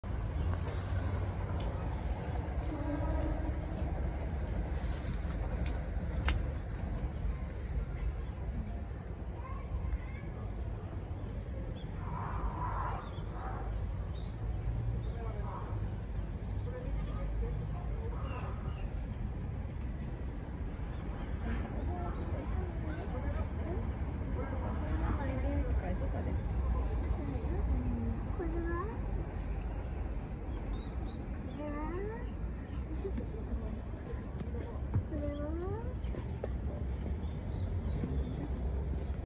20230225 1353 王子動物園 ジャガー マヤ
早く帰りた〜い!!と寝室に向かって鳴き出した。